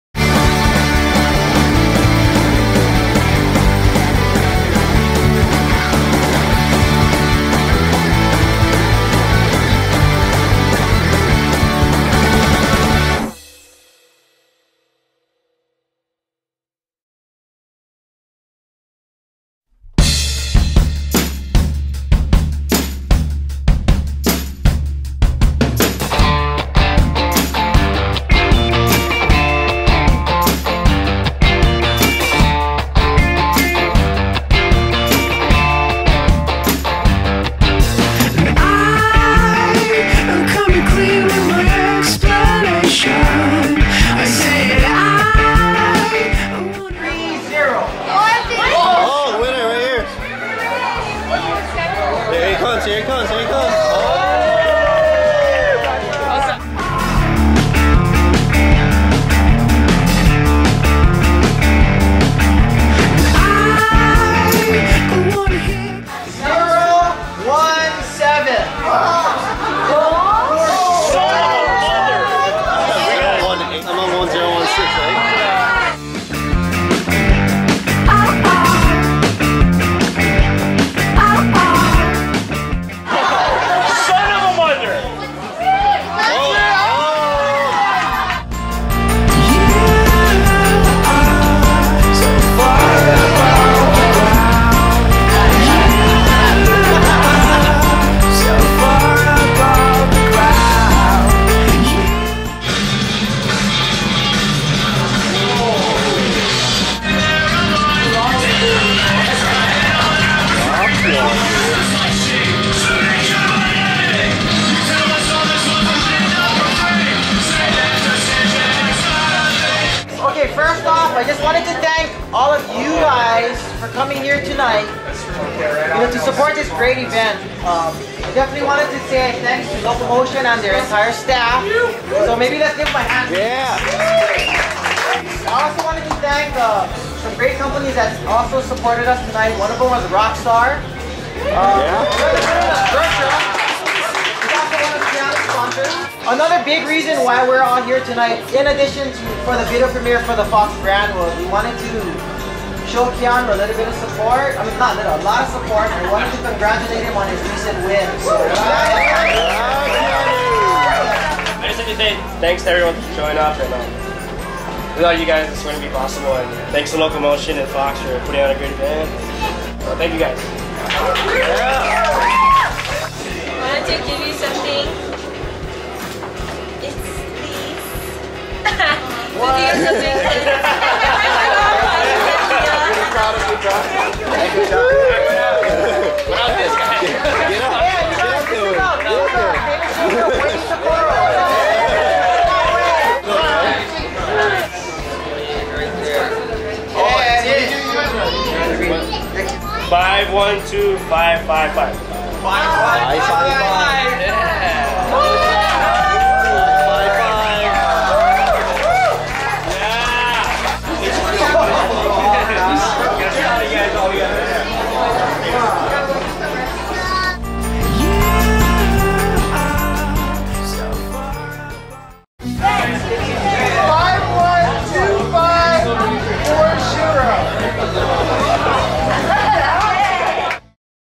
Fox Surf and Local Motion Congratulate Keanu Asing at the Showing of NoFilter at STABL (S4W36)
Fox Surf and Local Motion Congratulate Keanu Asing at the Showing of NoFilter at STABL (S4W36) from Novus Swell Featuring Hawaii's Next Generation Surfers. Like this? Watch the latest episode of Novus Swell Featuring Hawaii's Next Generation Surfers on Blip!

Fox Surf and Local Motion got together for an Hawaii premier of NoFilter the movie at STABL this past Friday. They also surprised Keanu Asing with baby Vans shoes to go with his giant Vans from recent win at Vanapos;s Pro Jr event in Virginia Beach. Free surf movie, food, and tons of free goodies for kids.

Thank you Fox and Local Motion for putting an amazing event!